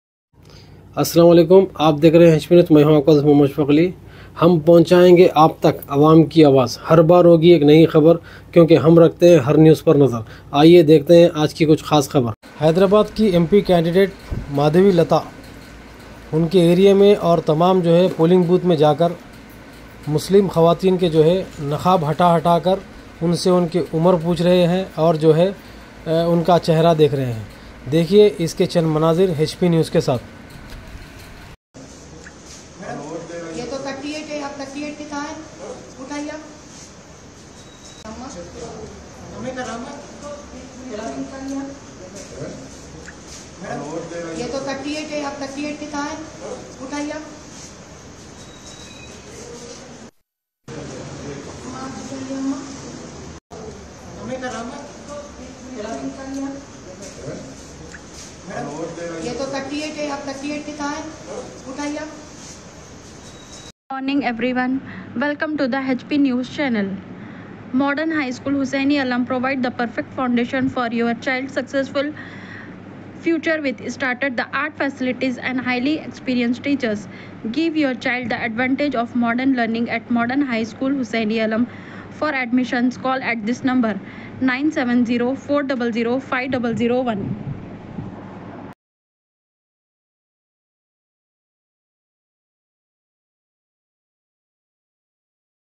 MP CANDIDATE MADHAVI LATA POLLING BOOTH MAIN MUSLIM KHAWATEEN KO NAQAAB UTHA KAR CHECK KAR RAHI HAI?
video MP CANDIDATE MADHAVI LATA POLLING BOOTH hindi MAIN MUSLIM hyderabad telangana KHAWATEEN KO aimim NAQAAB UTHA KAR CHECK KAR RAHI HAI?...HP NEWS CHANNEL